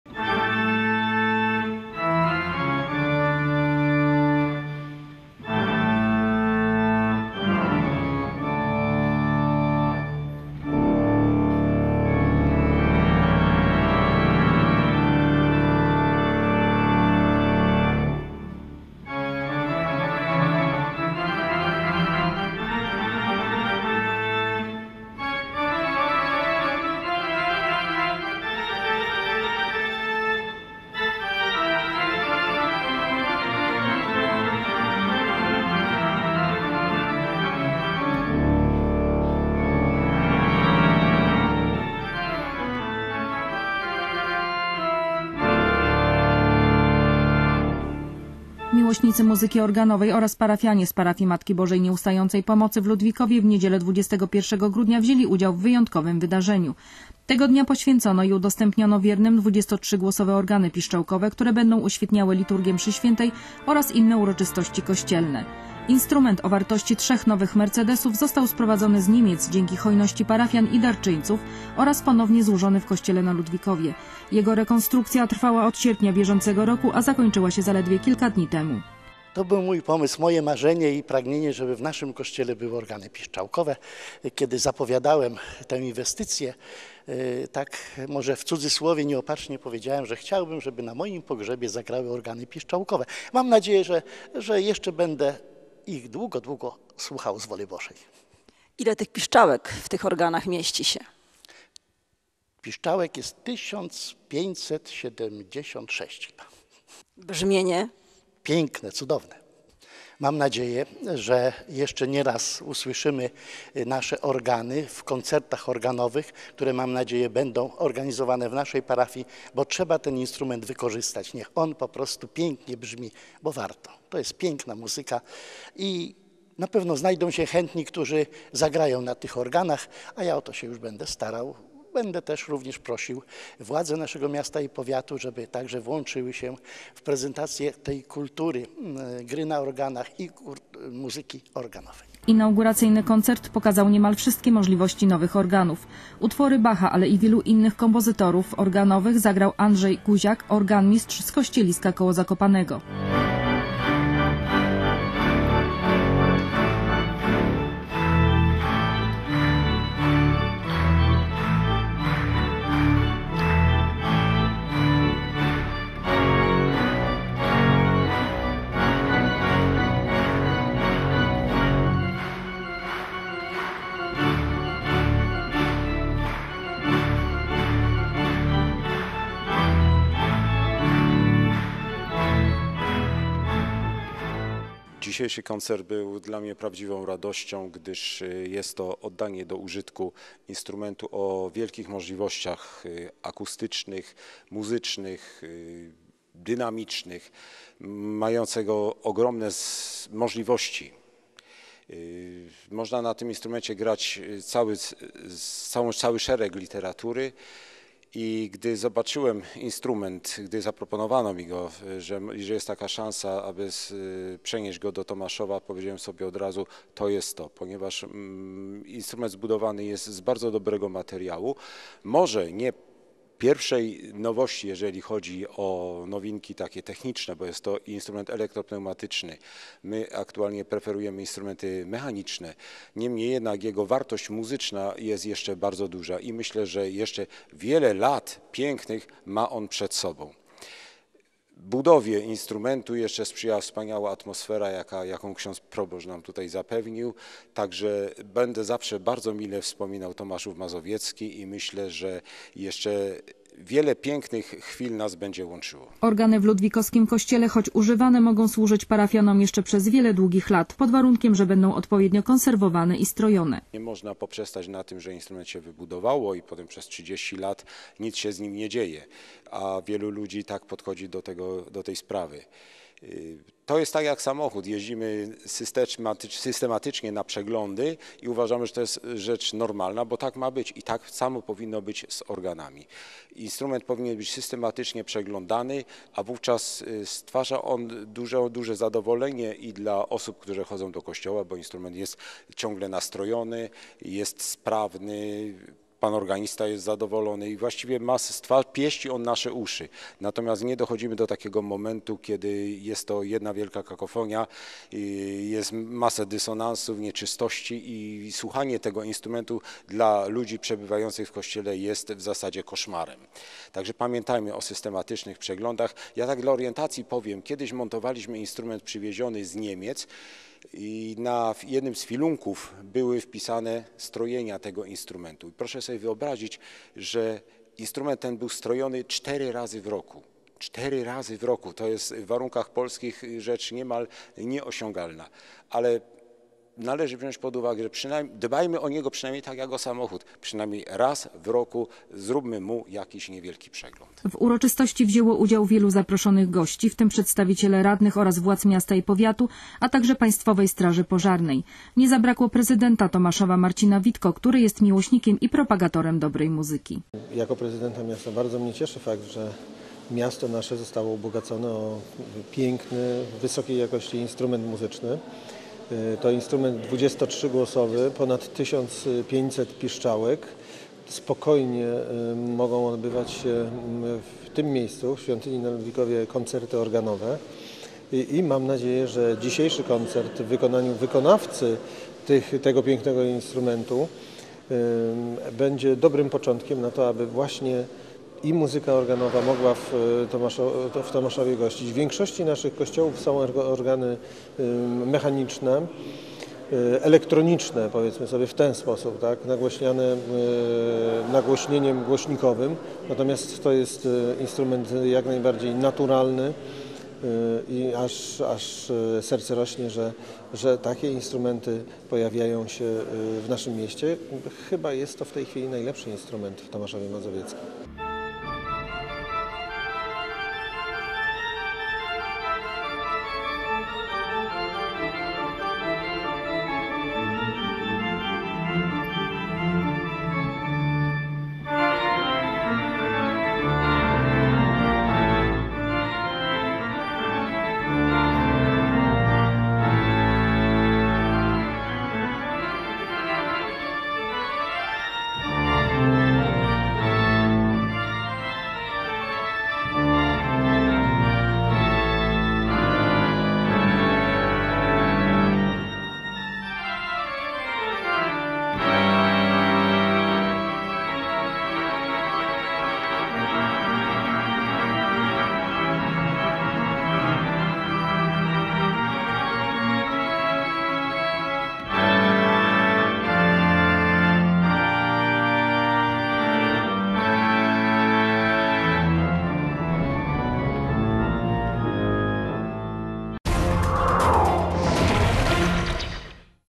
Organy w Parafii Matki Bożej Nieustającej Pomocy
Parafia Matki Bożej Nieustającej Pomocy w dzielnicy Ludwików w Tomaszowie Maz.,wzbogaciła się o niezwykle cenny element, a mianowicie organy, które zostały zakupione dzięki hojności parafian i darczyńców, i przywiezione z Niemiec. Organy składają się z 1576 piszczałek i mają "przepiękne brzmienie," jak mówi proboszcz Parafii ks. Zbigniew Kowalczyk, i dodaje, że jego marzeniem było mieć organy w kościele, bowiem muzyka organowa jest niezwykle piękna. Ksiądz Zbigniew już zapowiada organizowanie koncertów i zapraszanie wybitnych wykonawców muzyki organowej. Inauguracyjny koncert zagrał Andrzej Guziak, organmistrz z Kościeliska k/ Zakopanego, który również montował te  organy. Dłuższą relację z uroczystości poświęcenia organów i fragmenty koncertu nadamy wkrótce.